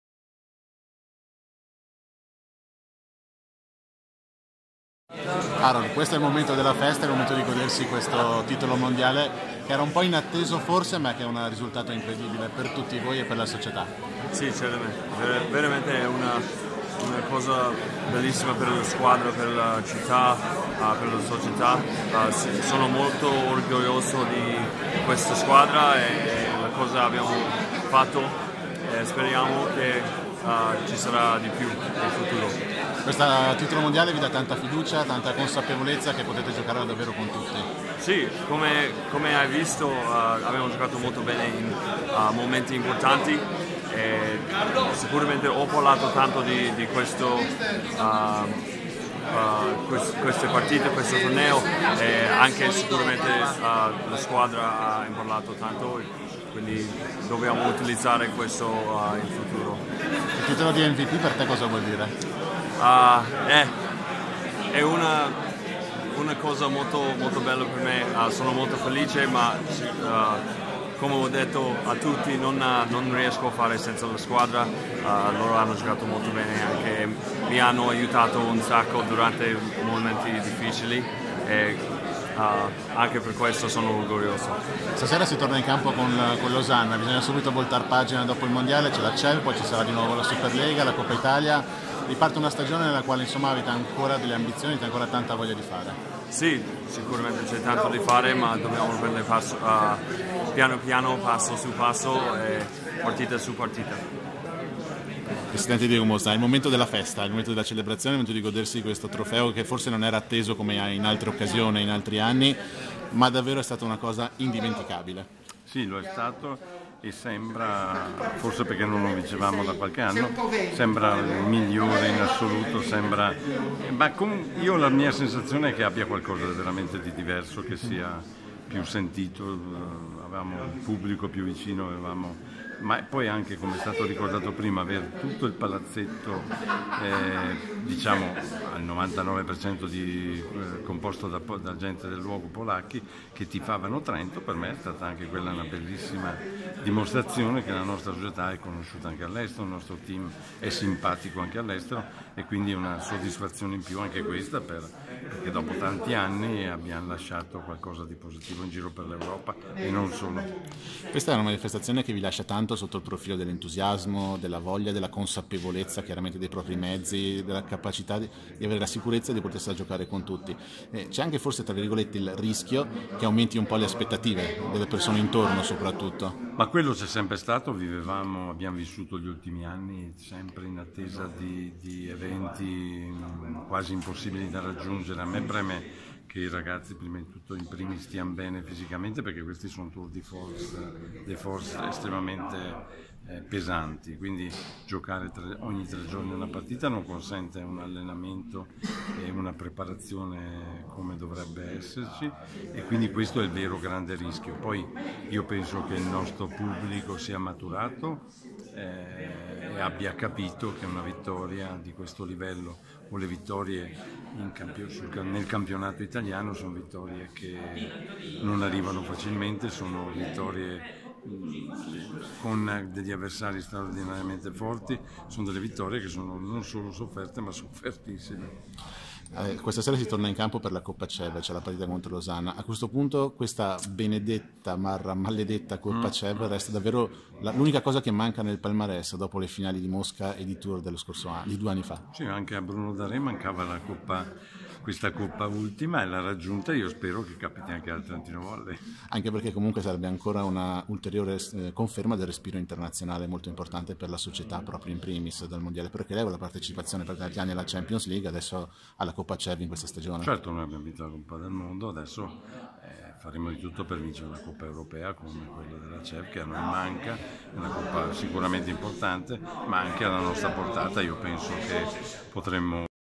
Aaron Russell e Diego Mosna raccontano il Mondiale per Club 2018
Le nostre video interviste ad Aaron Russell e Diego Mosna, schiacciatore e presidente della Trentino Volley, dopo la premiazione in Comune per ringraziare la squadra del Mondiale per Club 2018 conquistato in Polonia